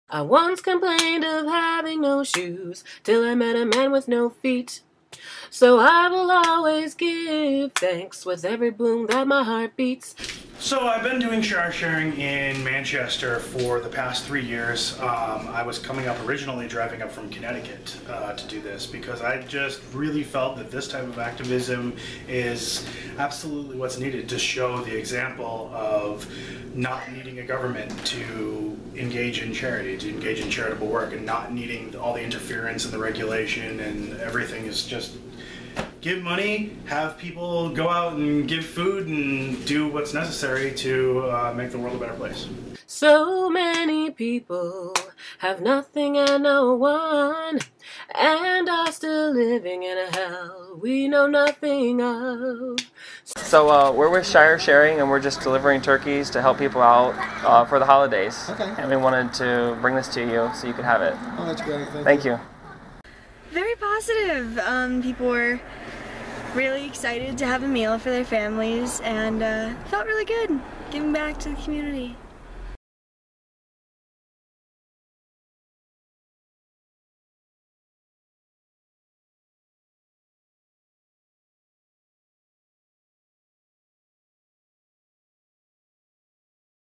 Shire Sharing Keene
On November 23, 2014 Shire Sharing Keene donated meals to 22 families in the Monadnock region.
Thanks to all who donated time, money and/or supplies for this effort.